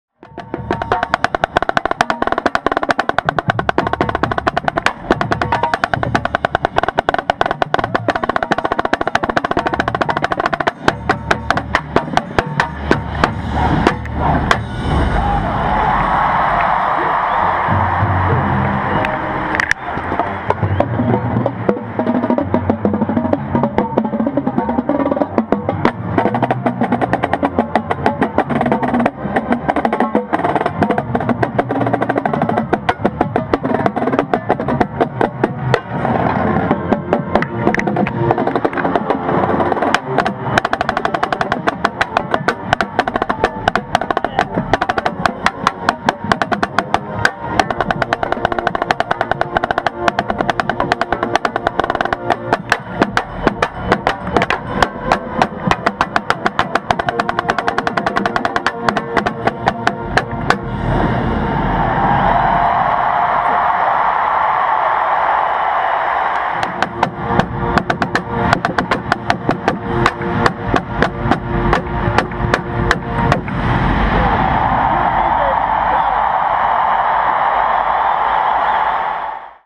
Snare drum falls off during DCI Finals
My snare drum came unhooked during my age-out finals run with Carolina Crown. Huge thank you to the judge Allan Kristensen for saving my performance! I'm guessing it came loose at the last note of the drum feature, and then was barely sitting in the hooks. While moving back field, it slipped out.
I will never forget the feeling it sent through my body as it fell off, and my reaction as I saw it hit the ground in front of me. I thought Max, the quad player was about to trip over it. Luckily he missed it by a hair! My reaction was to keep marching because muscle memory had already taken over, as you can see I brought my sticks up because it was part of muscle memory I suppose. I remember seeing AK pick up my drum, and hearing him yell "Yo!" to my left. He was on my left, and handed it to me in the perfect position for me to grab and hook onto my carrier right before a huge drill move.
This was a memorable age out to say the least.

-Ryan Hall